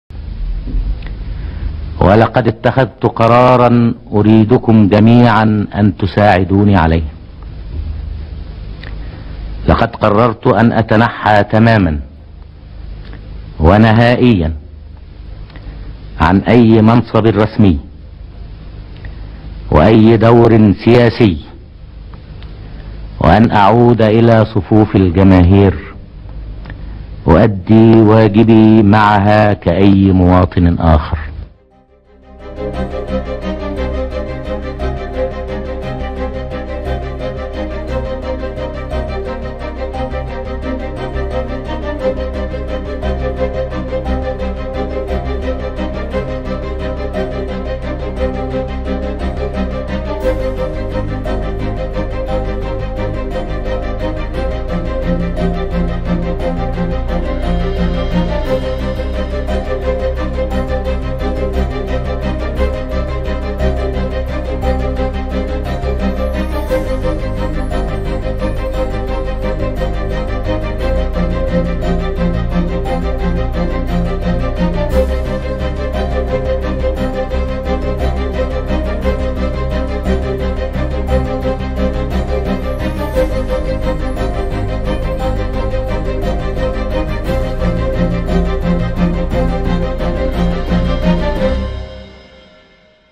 النكسة مستمرة
حرب 6 أيام آثارُها مستمرة منذ 55 عاماً.. ذكرى النكسة بعد هزيمة الجيوش العربية أمام إسرائيل